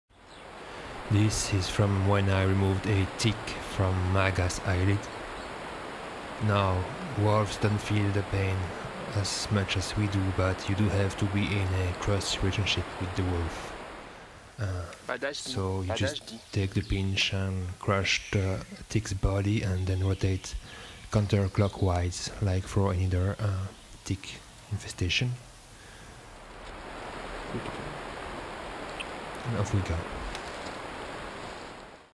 How to remove a tick from a wolf's eyelid (And lo Leviathan behind the scenes)
How to remove a tick from a wolf's eyelid